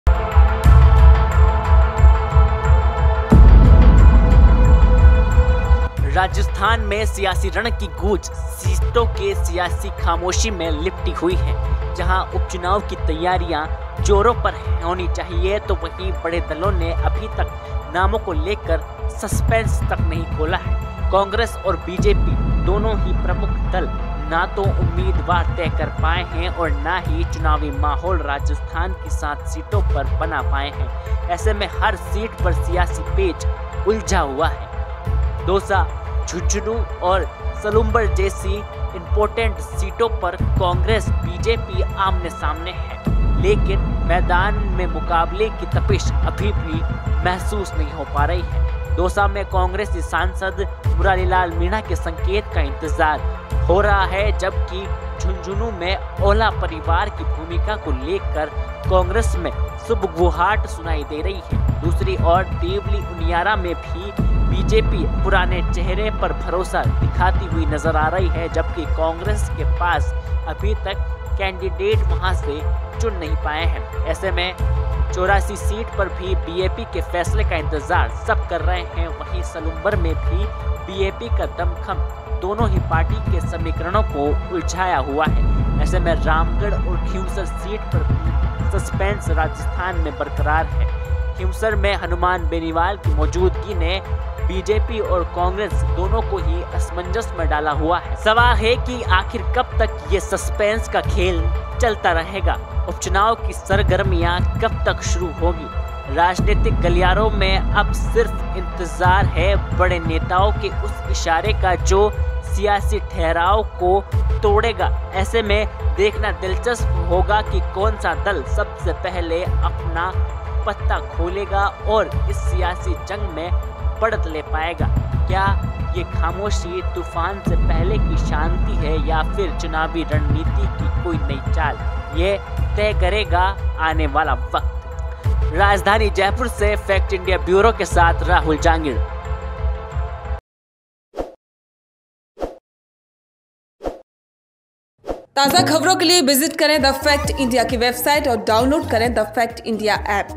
राजस्थान उपचुनाव में क्यों फंस रहा पेच? | Sachin Pilot | Rajkumar Roat | Hanuman Beniwal | TFI
Anchor/Reporter - Rahul Jangid